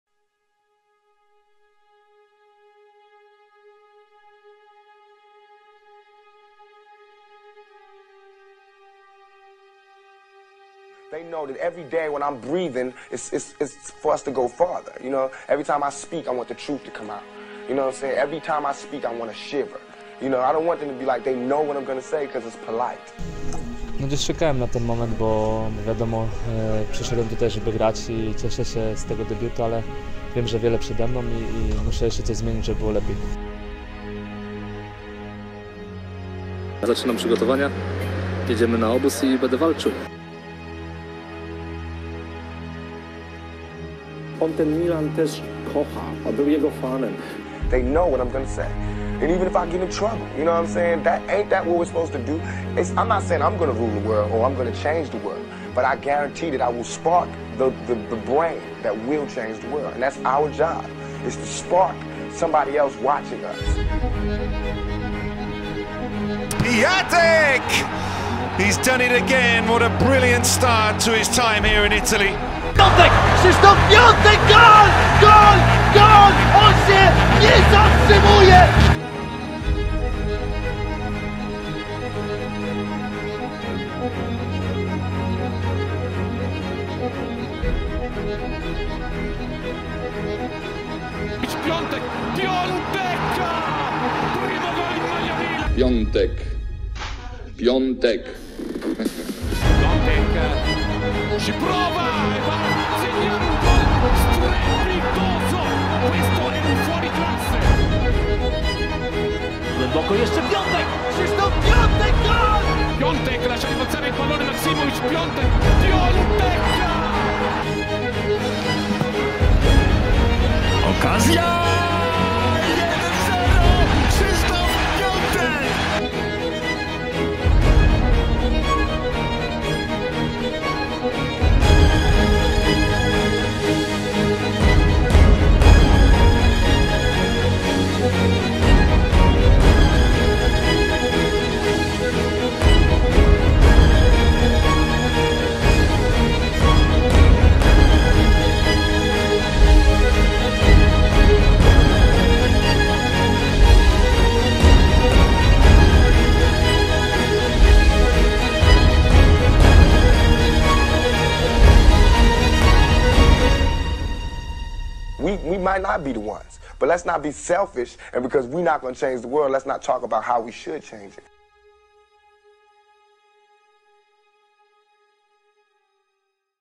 The Story of Krzysztof Piatek | Striker | AC Milan | Tribute
Tribute video to Krzysztof Piatek from AC Milan
All his best moments and goals